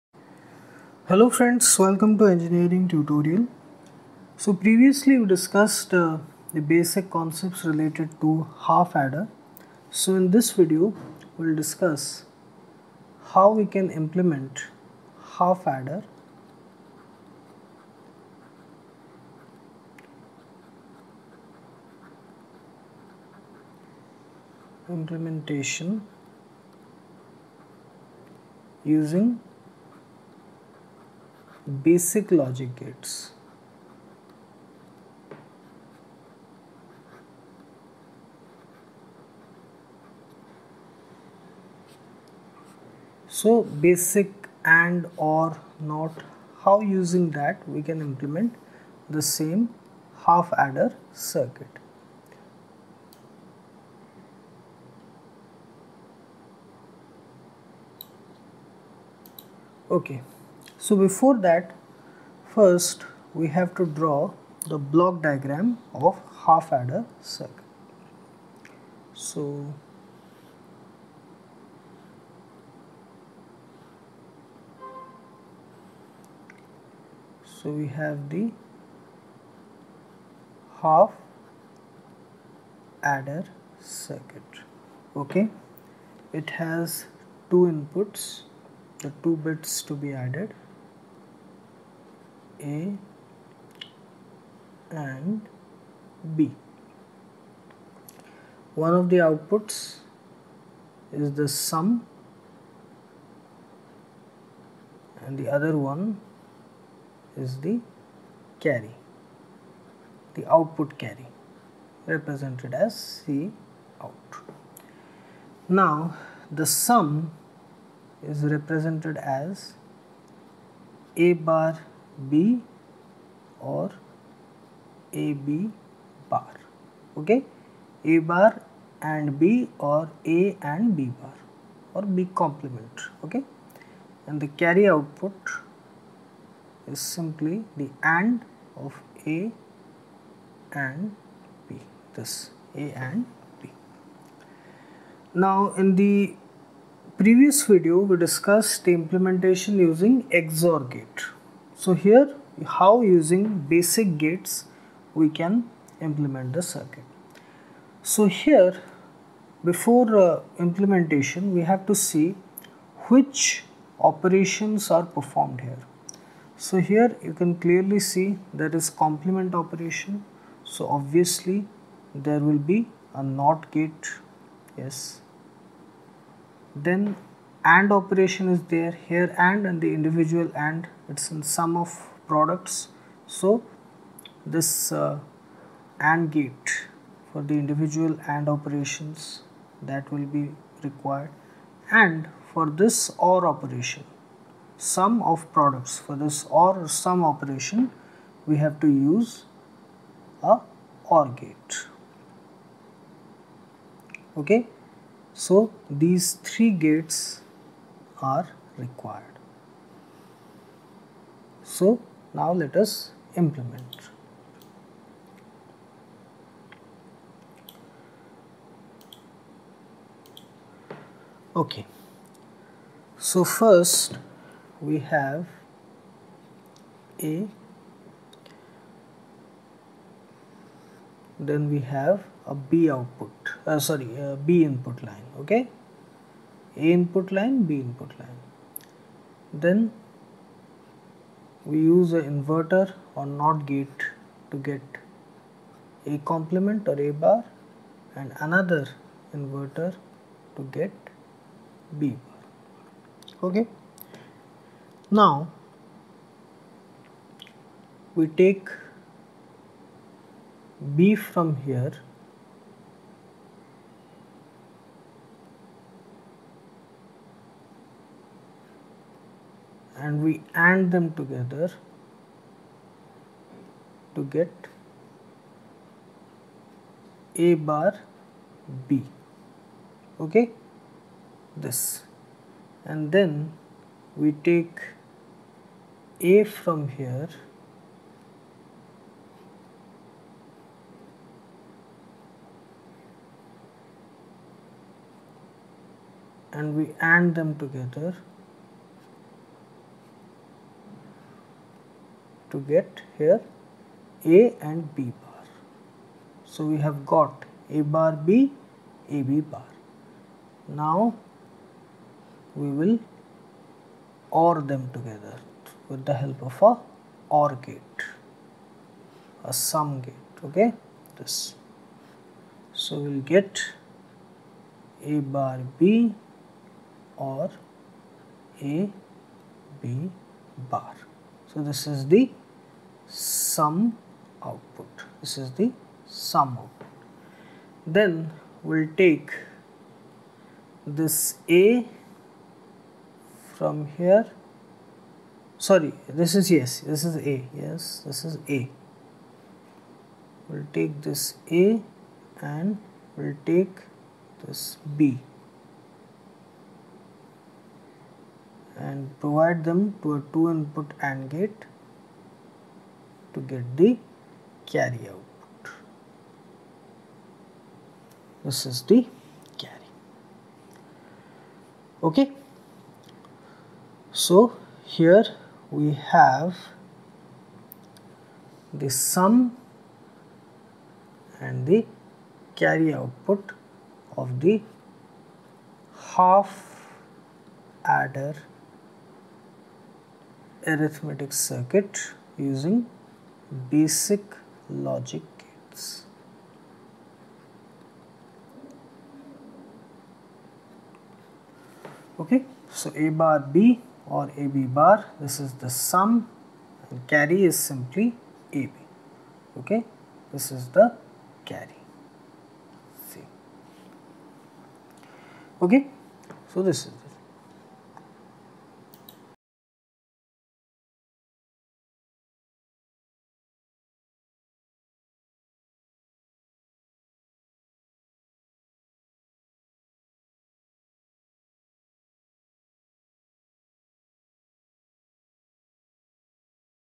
Half Adder | Using Basic Logic Gates | Digital Electronics
In this video, we are going to discuss some basic concepts related to Half Adder implementation using Basic Logic Gates.